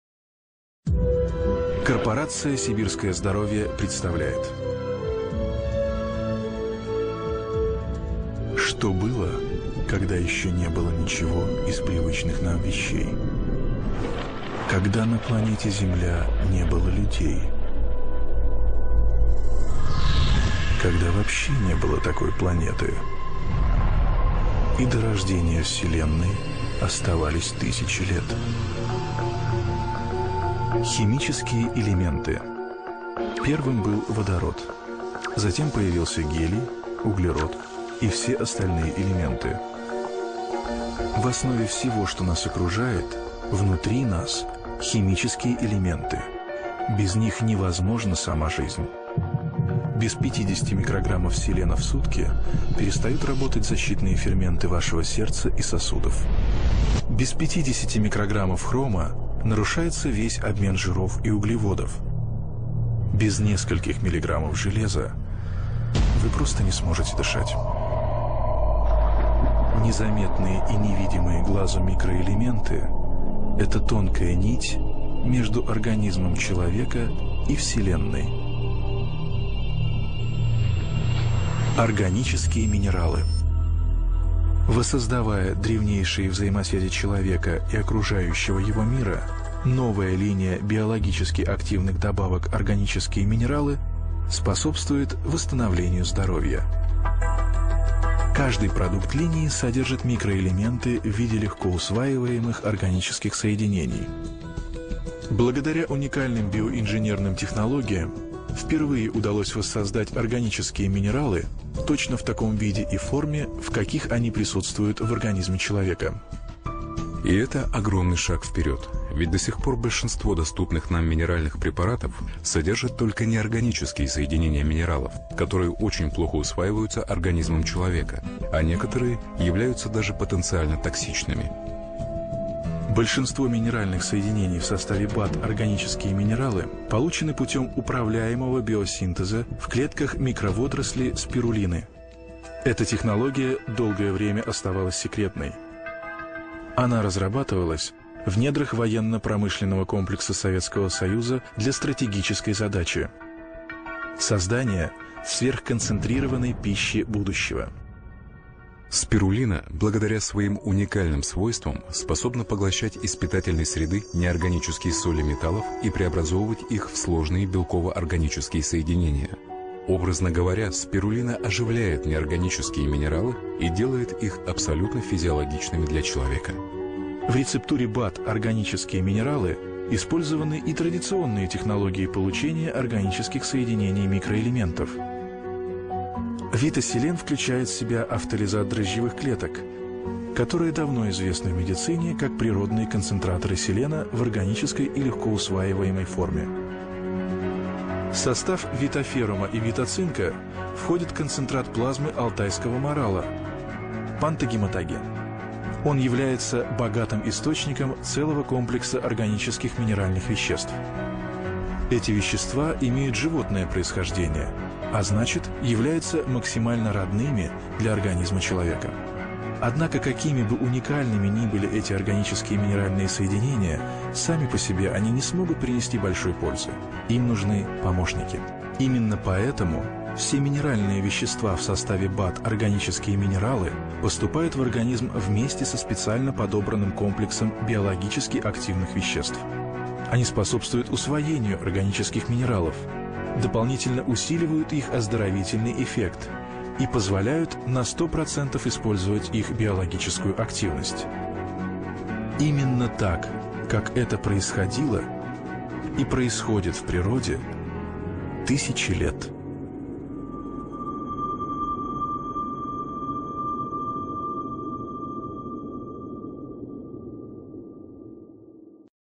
Органични минерали
Серия БАД "Органични минерали" на Корпорация "Сибирско здраве":
ВитаСелен, ВитаФерум, ВитаЦинк, ВитаХром, ВитаКалций, ВитаМагнезий, ВитаЙод.
С продуктите и бизнес-възможността на Корпорация "Сибирско здраве" можете да се запознаете на нашия сайт Повече информация за бизнеса

Превод и субтитри: П&А Аненкови